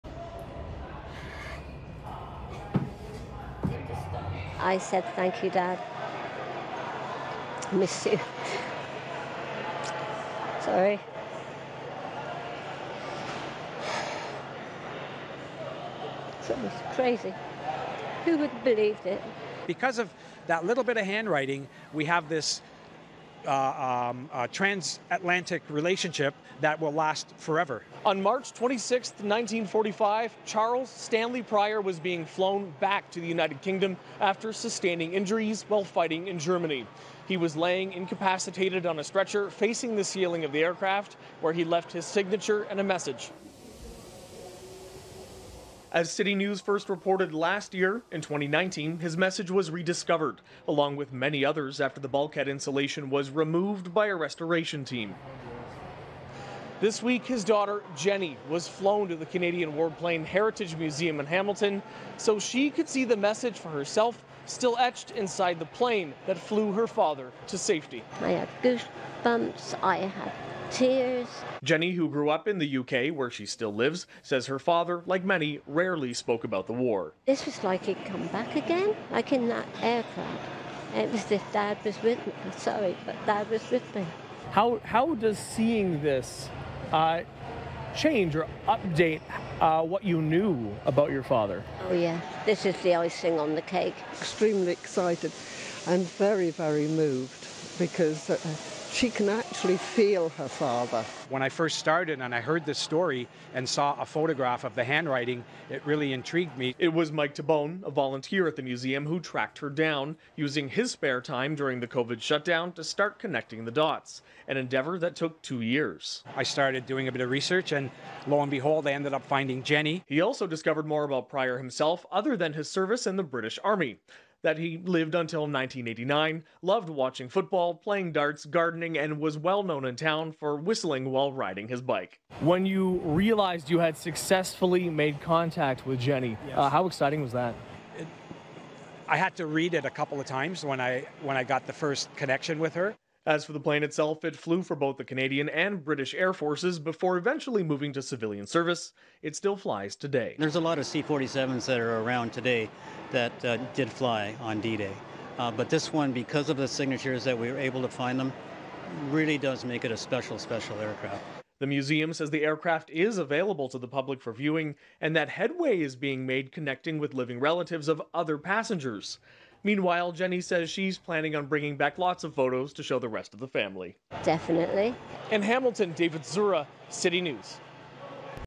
Uncovering messages long-hidden in war plane
A U.K. woman has crossed the ocean to see a hidden message written by her father inside a historic war plane. David Zura explains.